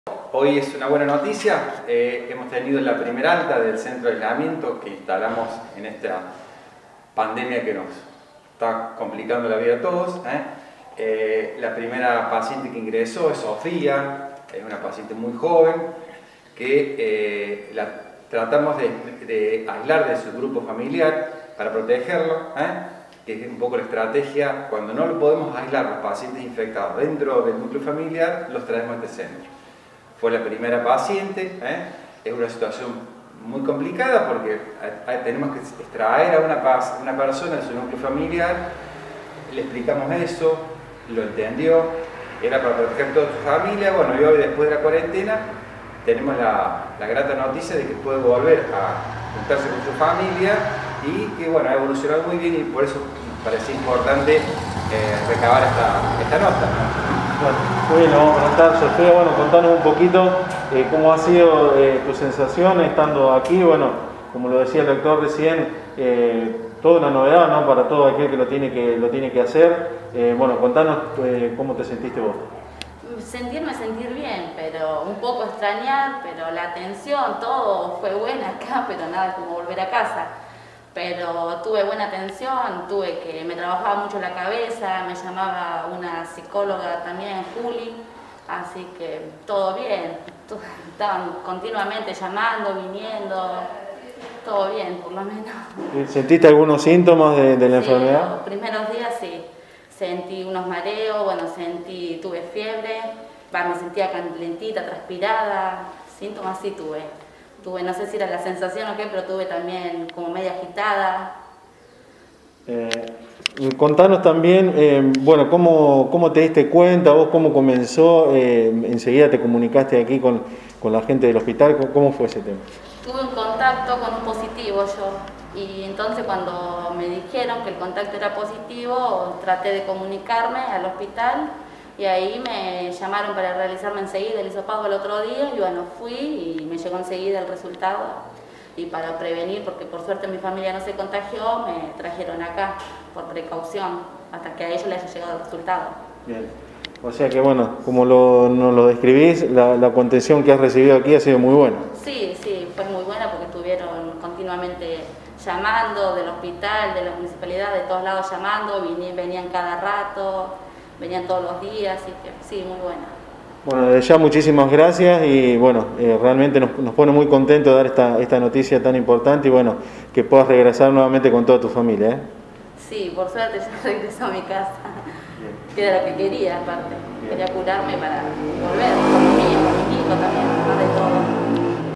PRIMERA PACIENTE CON EL ALTA MÉDICA QUE SE ENCONTRABA EN EL CENTRO DE AISLAMIENTO
En la mañana de este martes recibió el alta Sofía, primer paciente recuperada que se encontraba en el Centro de aislamiento. 👏👏💪💪